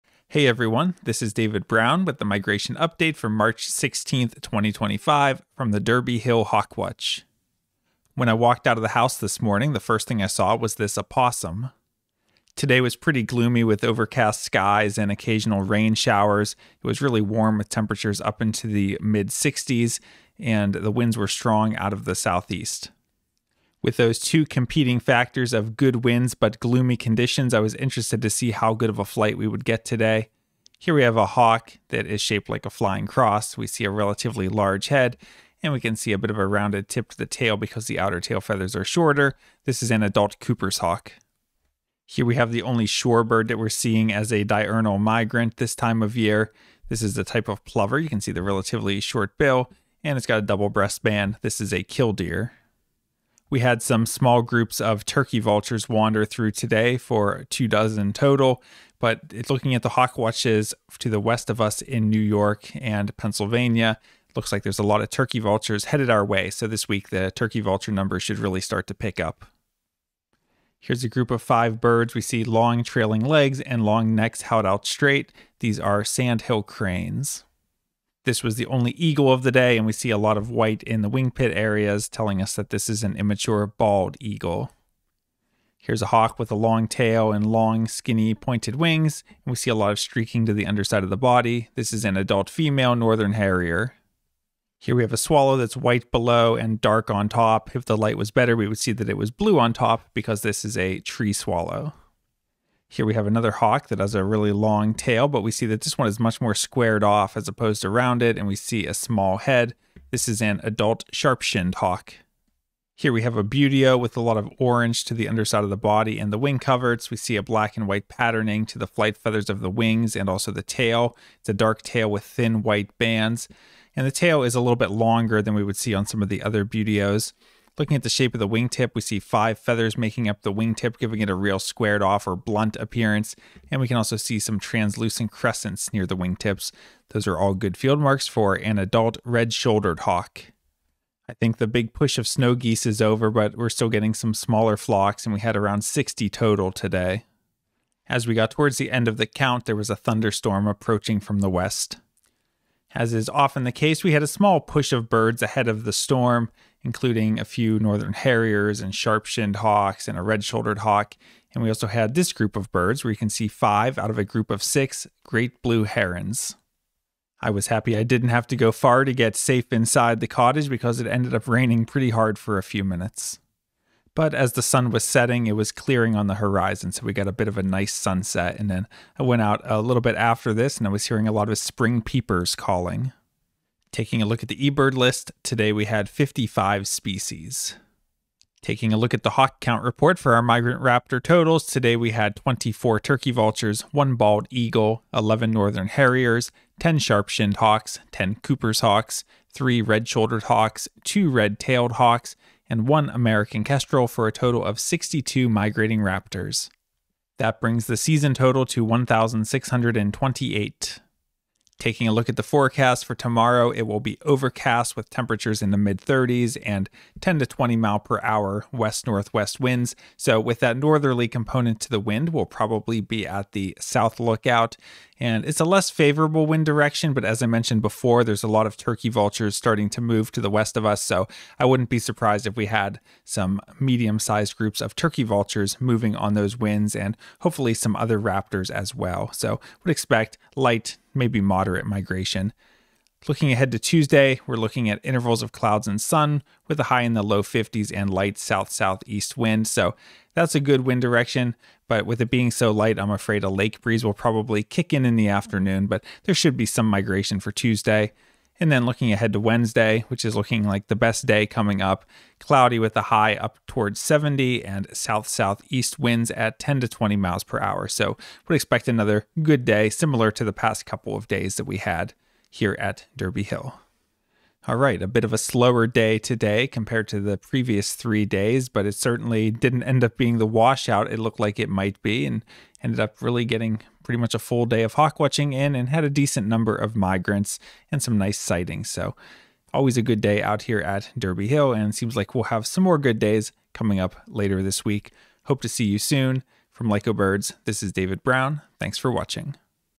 [16] Decent Migration Despite Gloomy Conditions and Rain at the Derby Hill Hawk Watch, 3/16/25
Hawk Watcher David Brown gives the daily summary of the raptors and bird sightings at the Derby Hill Hawk Watch. The Derby Hill Bird Observatory is located in Mexico, New York, USA.